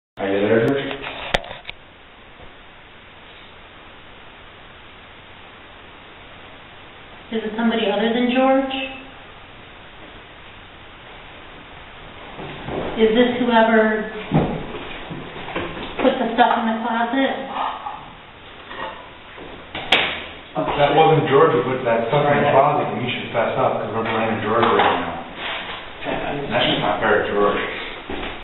Can't Tell You George's Piano Room
Video was taken at Rolling Hills Asylum - Admin floor - brick building - george's room.

We were asking about some mystrious spots in the closets.  We now know what caused those spots...  but sounds like somone couldn't or wouldn't tell us what was happening.

This is for the audio not the video...